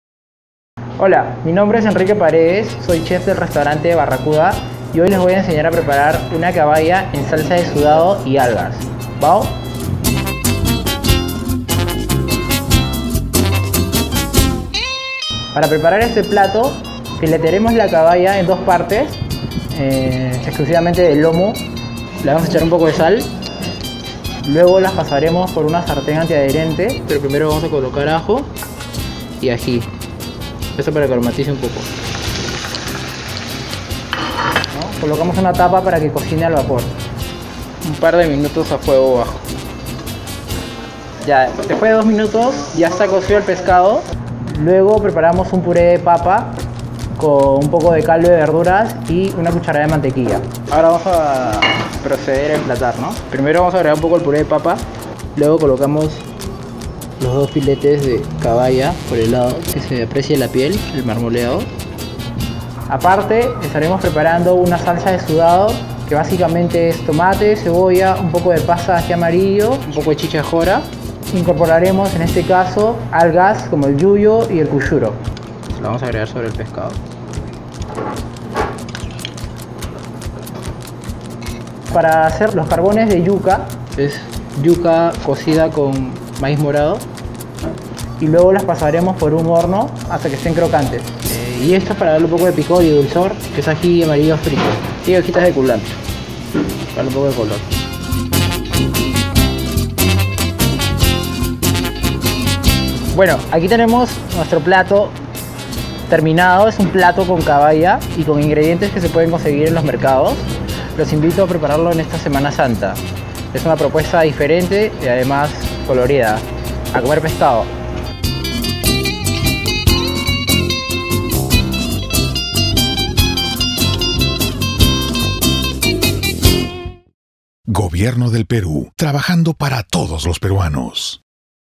Aprende a preparar "caballa en salsa de sudado y algas"
Aprende a preparar con el chef Enrique Paredes  de "Barra Khuda".